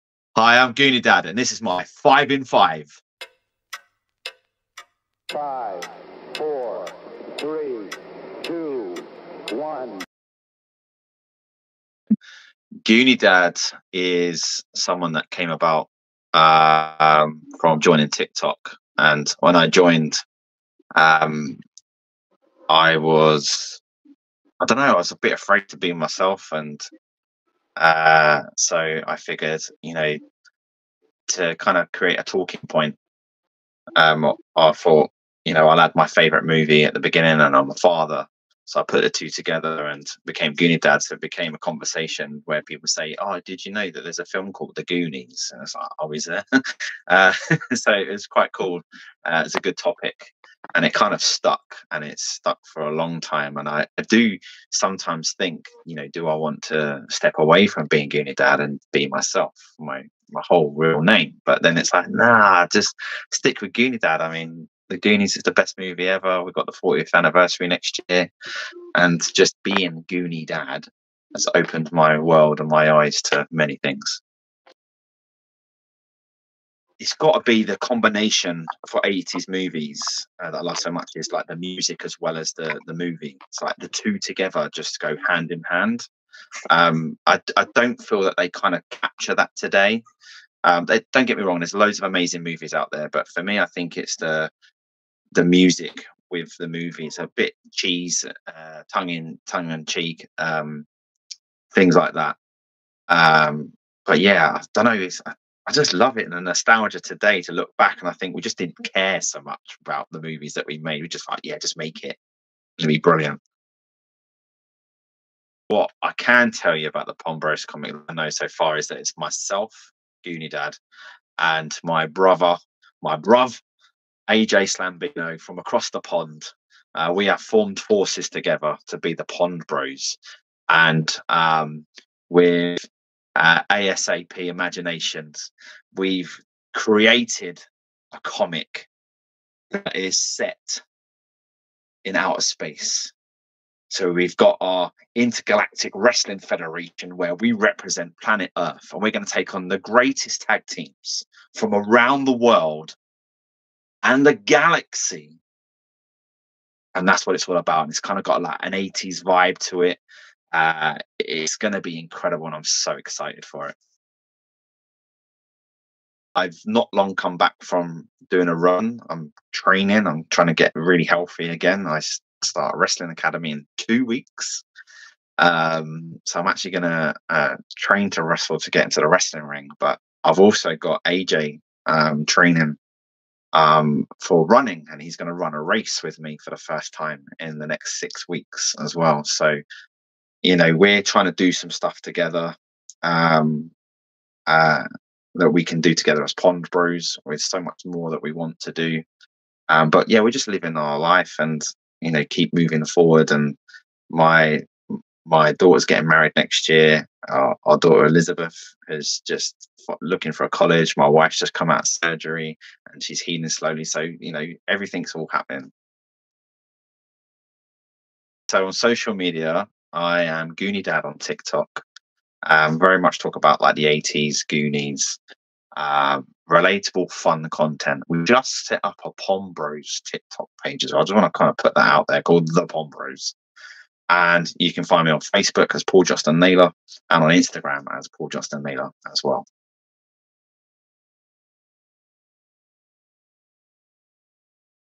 Creator Spotlight - Paul Johnston-Naylor AKA GOONIEDAD 5in5 Full Episode
The creator 5in5's are back!
This time we're grabbing 5 with Paul Johnston-Naylor aka GOONIEDAD!